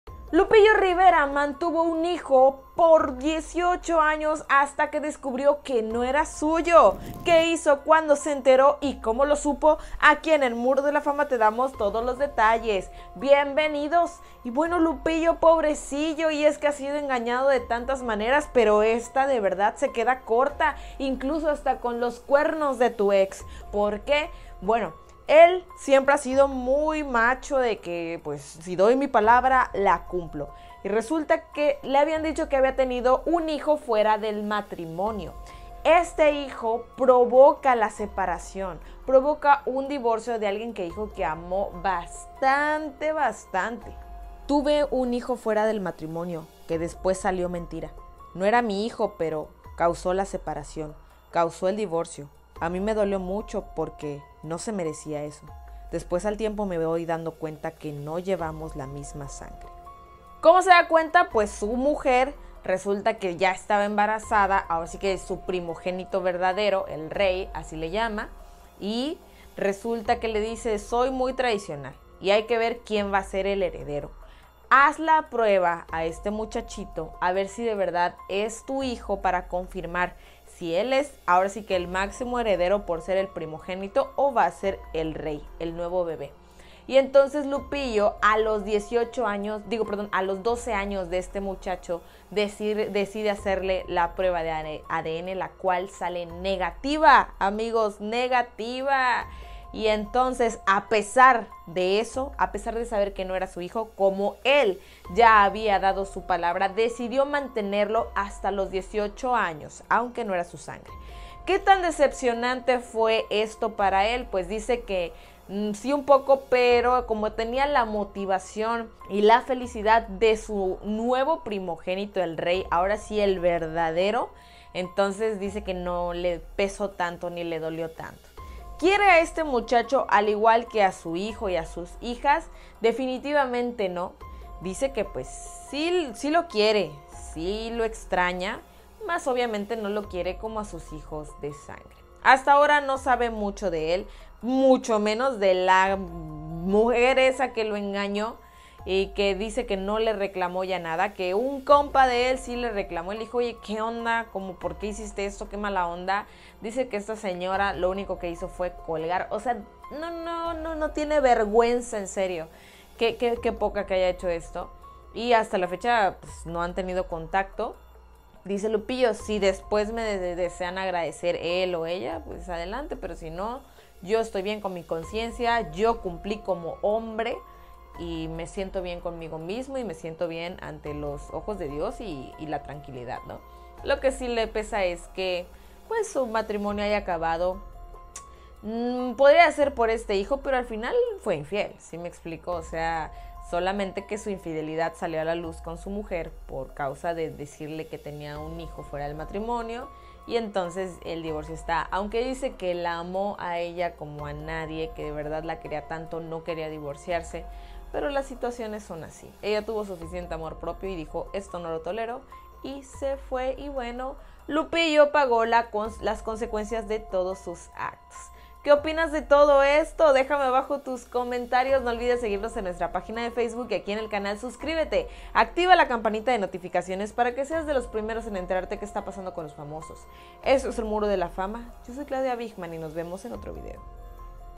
😱👎 𝗗𝗘𝗩𝗔𝗦𝗧𝗔𝗗𝗢 Lupillo Rivera 😭 Esto Fue Lo Que Descubrio: Aqui Toda la verdad
De Esta Manera Le Mintieron a lupillo rivera y quedo super devastadooo